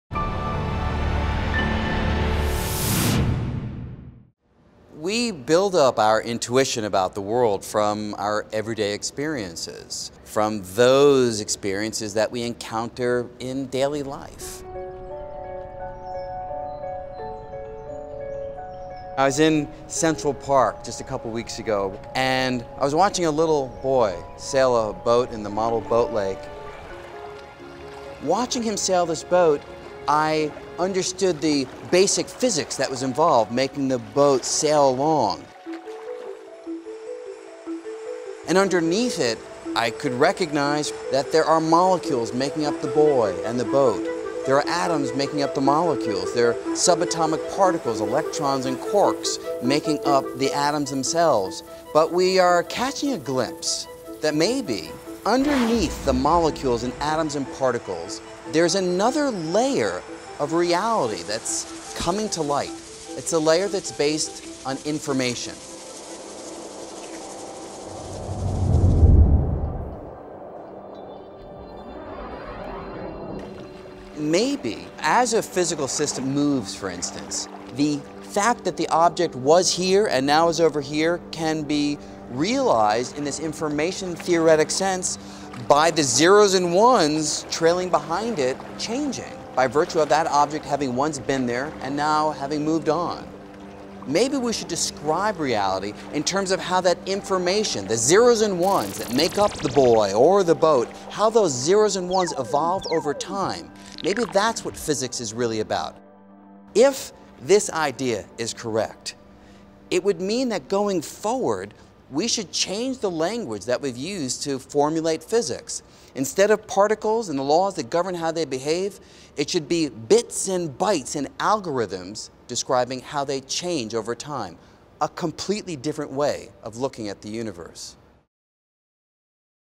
Is the Universe Digital?
When scientists first suggested that universe could be compared to a computer, many balked at the concept. They pointed to the ancient Greeks who envisioned a universe of spheres, and later, those who compared it to the workings of a clock. But what the research is suggesting is nothing so literal. Or is it? Journalist John Hockenberry sets the stage for this discussion with a clip featuring Brian Greene on the fundamental basis for this concept.